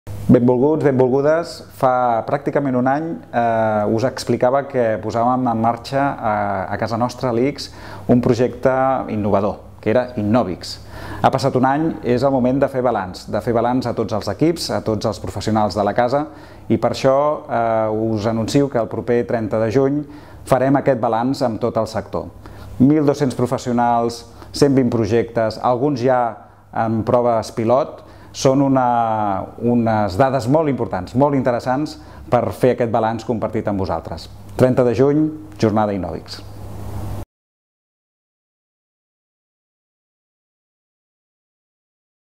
David Elvira et convida a assistir a la Jornada Innòbics del proper 30 de juny
El president de l'ICS, David Elvira, convida als professionals de la institució a assistir a la Jornada Innòbics, que se celebrarà a l'Auditori Axa de Barcelona el proper dia 30 de juny.
La sessió servirà per fer balanç del primer any del projecte d'innovació oberta de l'ICS, que compta amb la participació de més de 1.200 professionals, que han proposat més de 130 idees innovadores.